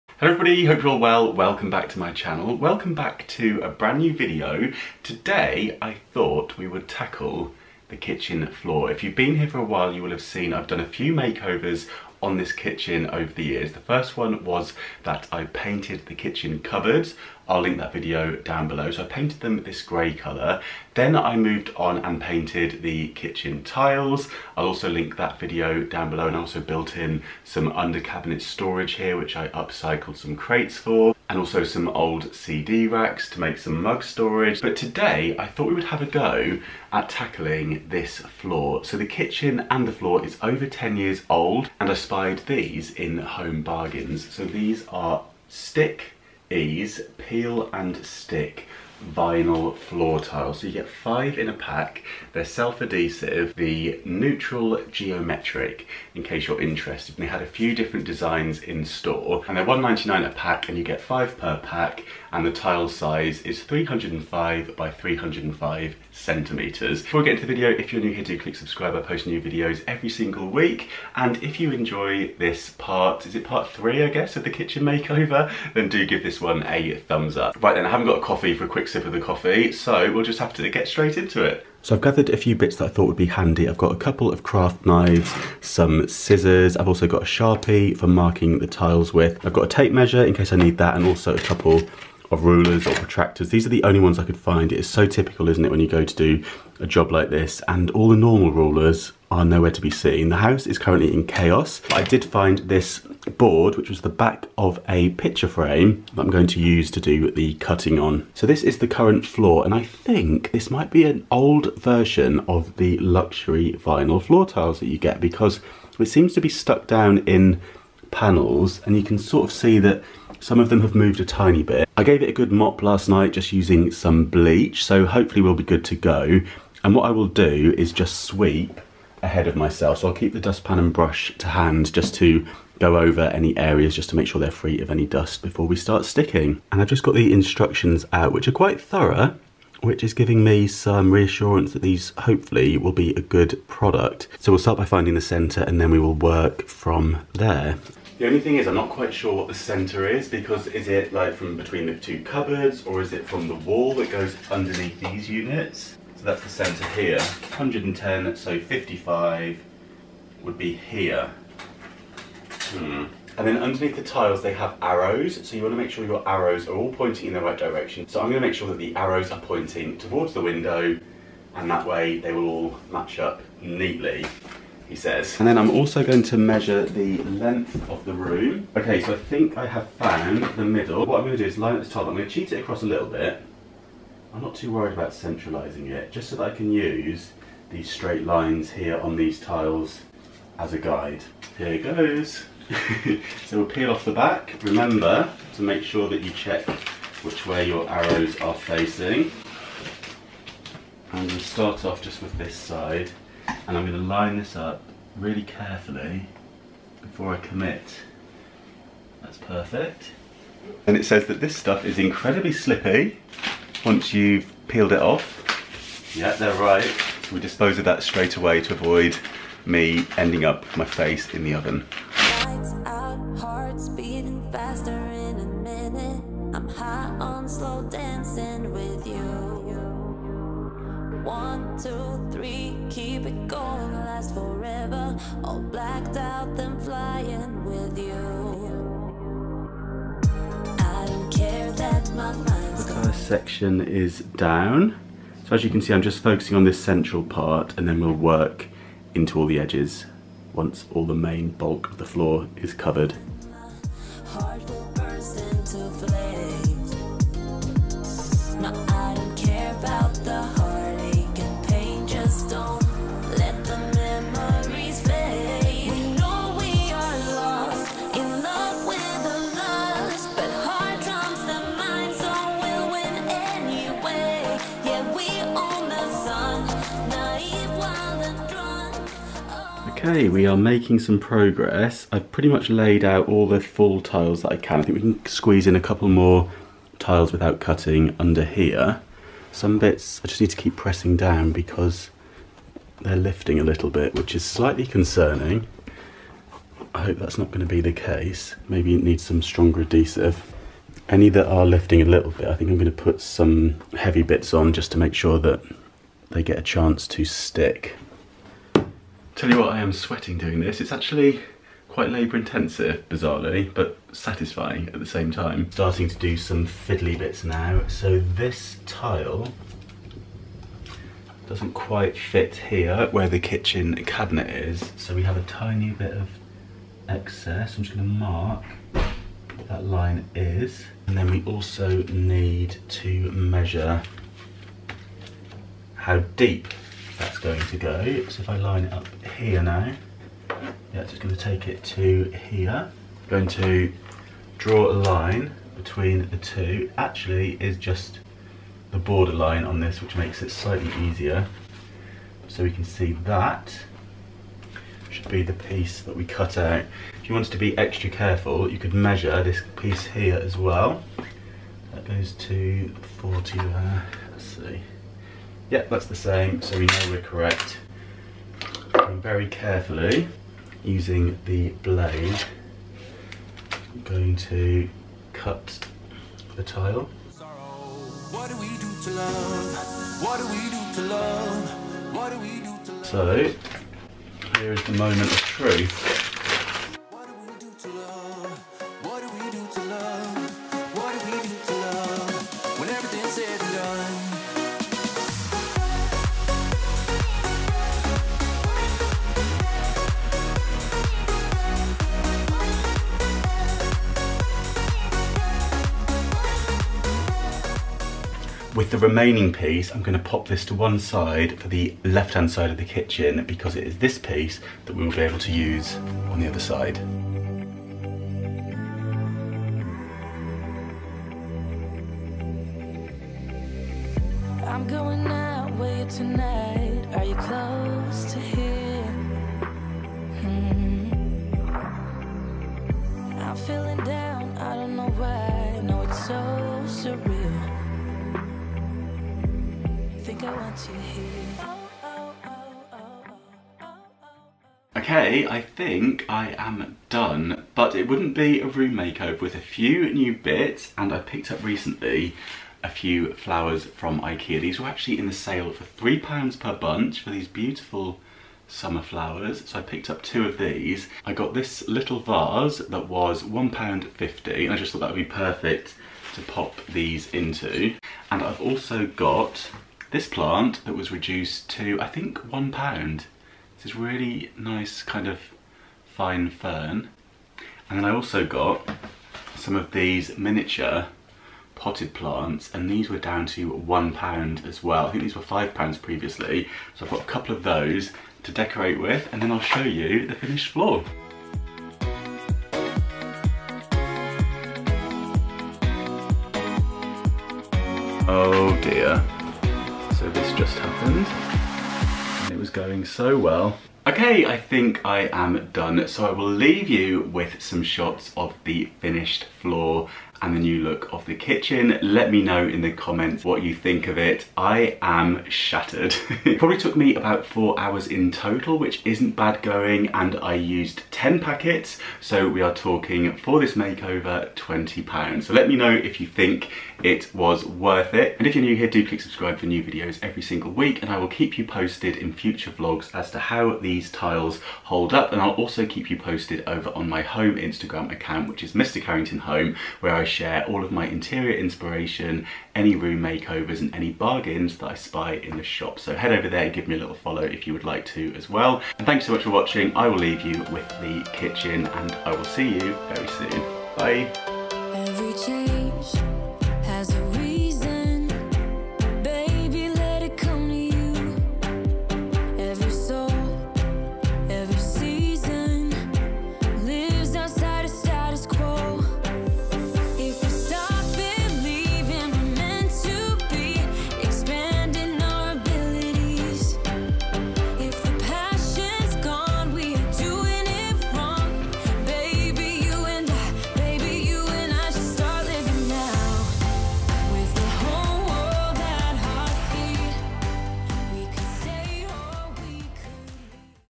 BUDGET KITCHEN FLOOR MAKEOVER USING HOME BARGAINS PEEL & STICK VINYL TILES | *£20!* | MR CARRINGTON
FILMING EQUIPMENT I USE:
*Camera I use
* Instagram Camera
* Instagram Lens